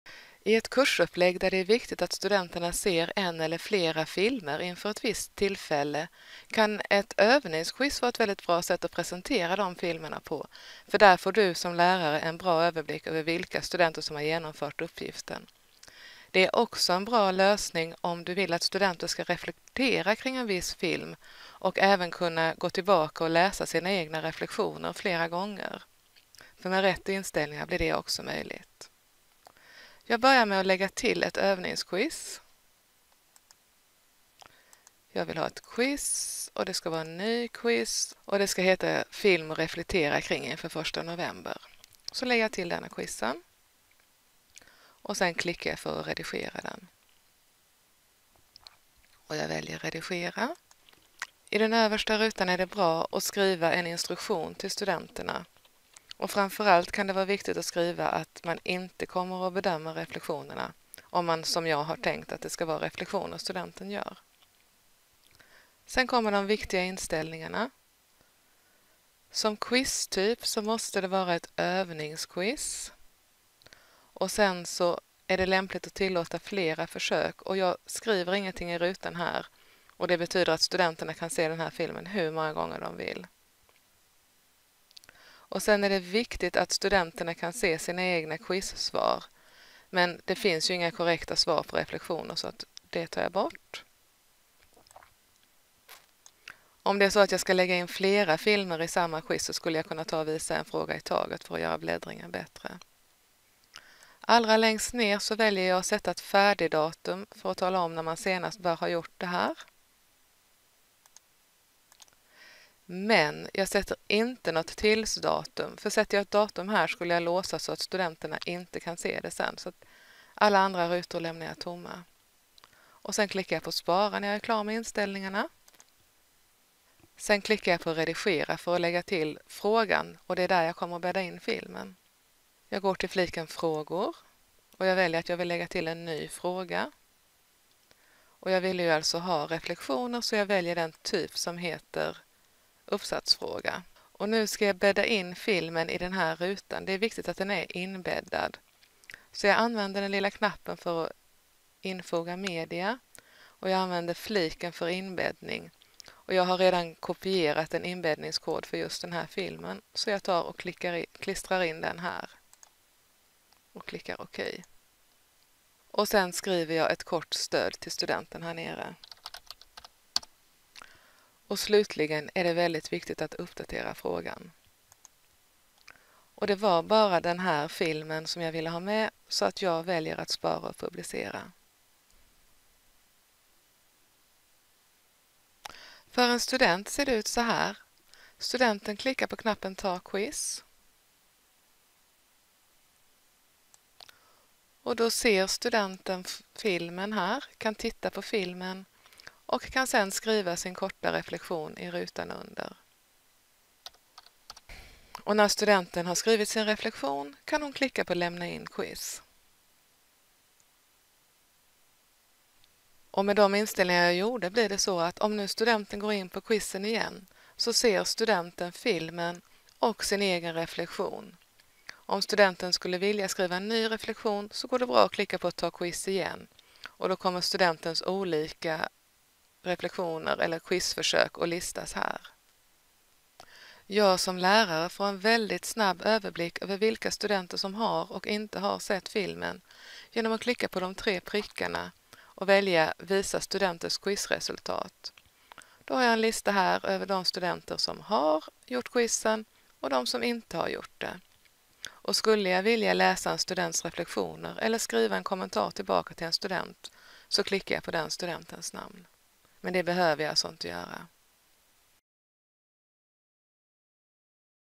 Canvas - filmreflektion i övningsquiz
Du kan använda ett övningsquiz för att få en överblick över vilka studenter som sett en viss film och/eller för att ge studenterna möjlighet att reflektera över filmen.